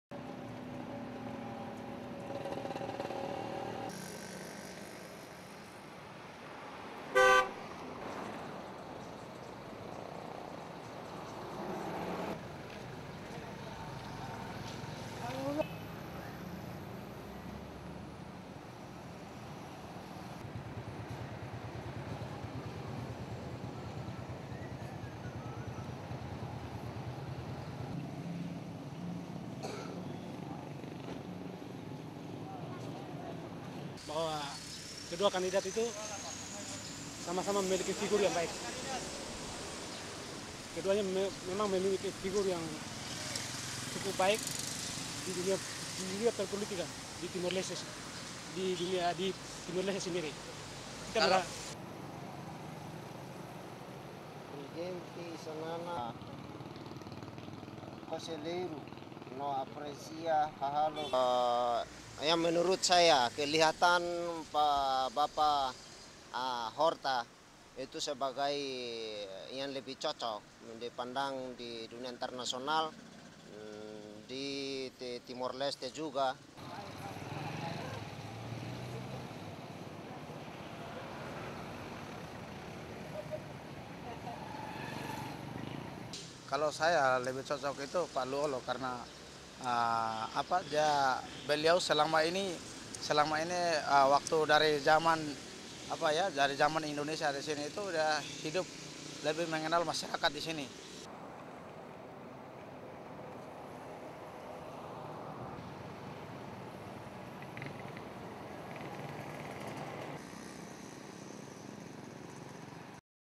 Vox pops, newspapers after partial results for first round of Presidential election
1. Traffic on street outside Government building
2. Sign on Governor's building
3. People getting off minibus
4. Various of men selling newspapers on street
5. Various of men reading newspapers
6. SOUNDBITE: (Indonesian) No name available, Timorese resident, Vox Pop:
"We are very happy that both candidates are very strong figures in the political arena of East Timor."
7. Man reading newspaper
8. Cutaway of newspaper
9. SOUNDBITE: (Indonesian) No name available, Timorese resident, Vox Pop:
"I think (Jose Ramos) Horta is the best one because he is well-known in the international community as well as East Timor."
10. Various of man selling phonecards on street
11. SOUNDBITE: (Indonesian) No name available, Timorese resident, Vox Pop:
"I like Pak Lu-olo. I think (Francisco) "Lu-olo" (Guterres) is the best to be elected because since the beginning he has been here. During the Indonesian era he lived in this country so he knows better the East Timorese people."
12. Various of harbour
13. Wide of lighthouse on headland
STORYLINE:
East Timor headed toward a run-off presidential vote Wednesday after partial election results showed the two frontrunners failed to get a majority
shattering hopes of ending political instability in the volatile, young nation.
Adding to tensions, five candidates claimed fraud.
With more than 70 percent of ballots counted on Wednesday, Prime Minister Jose Ramos-Horta, initially considered the favourite for the five-year post, was trailing Francisco "Lu-Olo" Guterres of the Fretilin party by 20,000 votes, the National Election Commission said.
A runoff was certain as each had less than 30 percent of the vote, far short of the 50 percent plus one needed to win outright.
Five candidates have already protested at the results, alleging in a letter to the electoral commission that figures had been manipulated.
The showdown between the two top vote getters, still uncertain, was scheduled for 8 May.
"We are very happy that both candidates are very strong figures in the political arena of East Timor," a Timorese resident told AP Television.
Others were more certain about who they wanted as their next president.
"I think Horta is the best one because he is well-known in the international community as well as East Timor," said one man.
Another thought Guterres should be elected, "because since the beginning he has been here. During the Indonesian era he lived in this country so he knows better the East Timorese people."
East Timor was heralded as a success in nation-building when it formally declared independence from Indonesia in 2002.
But it descended into chaos last year after then-Prime Minister Mari Alkatiri fired a third of the tiny army, provoking gunbattles between rival security forces that spiralled into gang warfare and looting.
At least 37 people were killed and some 155,000 fled their homes before the government collapsed.
Voters hoped that Monday's elections, which will be followed by a more-crucial parliamentary poll in June, would help end months of violence and instability.